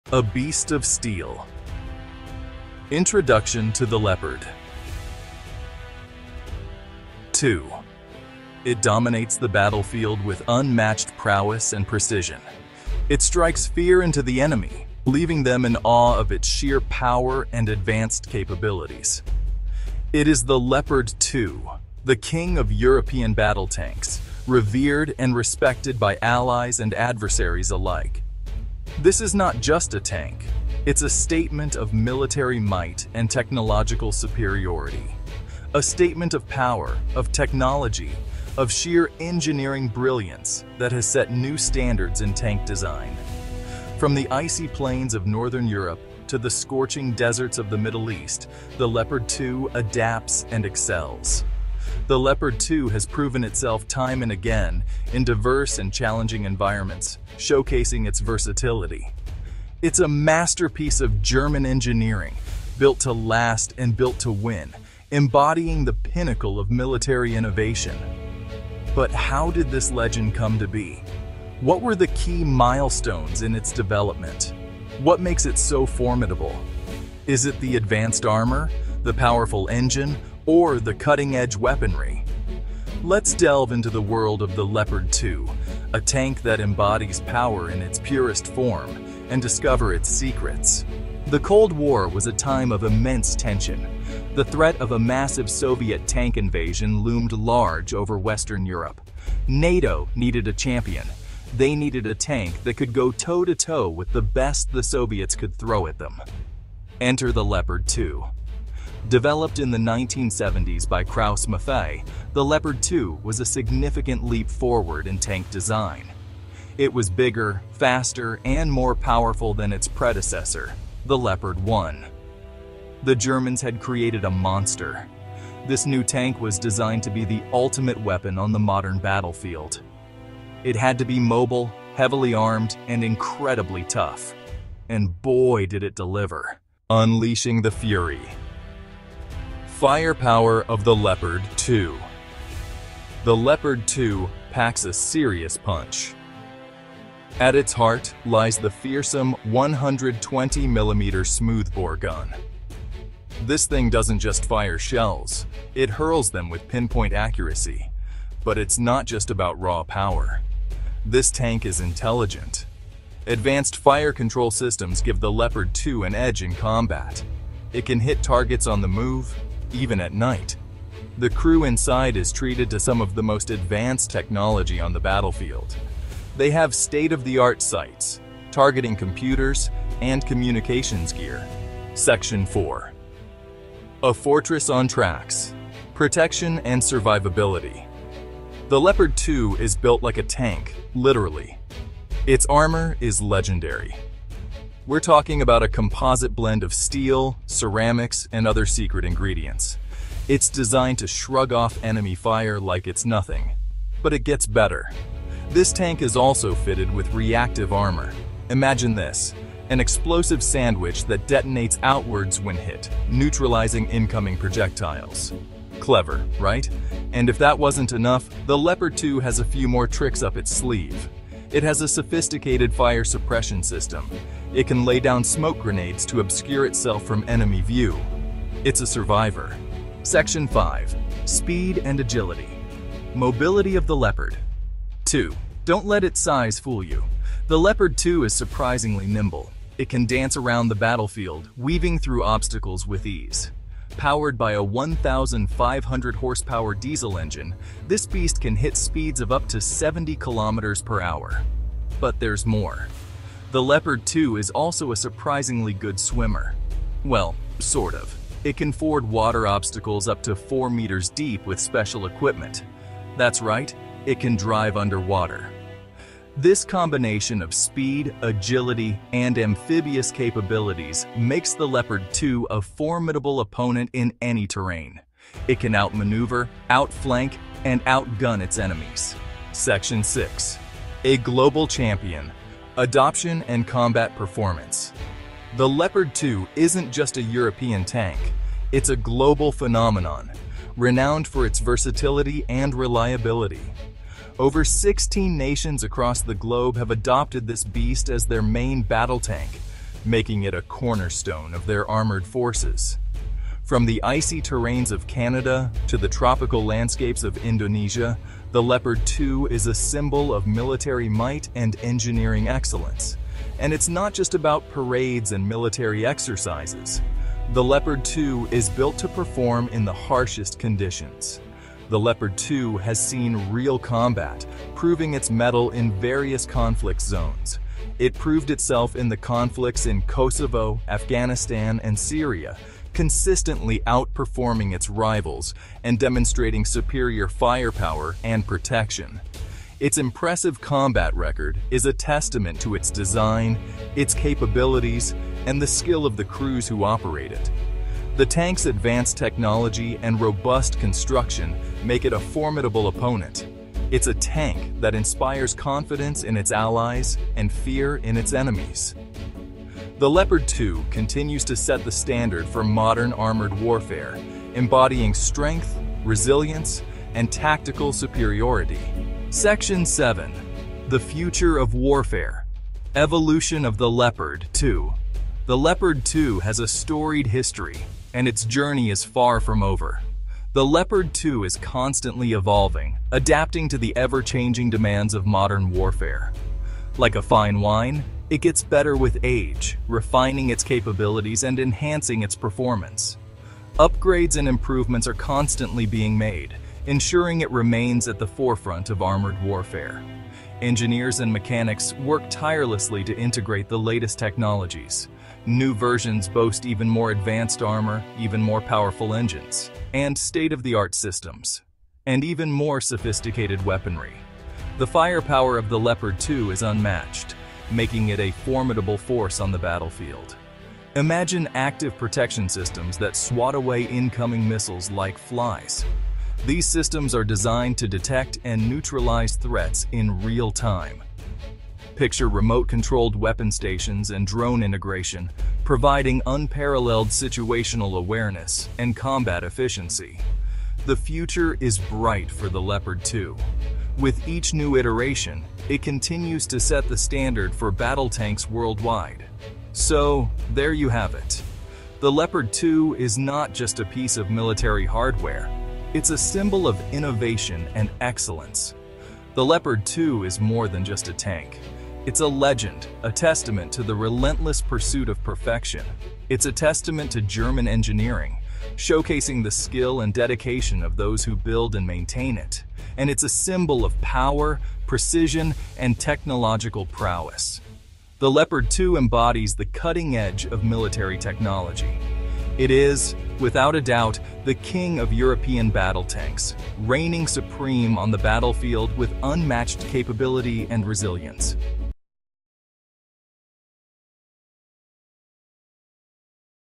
Germany Unveils the Most Powerful $3 Billion Laser Tank System
The Leopard 2 is Germany’s premier main battle tank, renowned for its firepower, armor, and mobility. Armed with a powerful 120mm smoothbore gun, it delivers unmatched precision and lethality on the battlefield. Its advanced composite armor and modular protection systems make it one of the most survivable tanks in modern warfare. With a top speed of 45 mph and exceptional off-road capabilities, the Leopard 2 can maneuver through the toughest terrains. Operated by over 20 nations and continuously upgraded, it remains a dominant force in global military operations.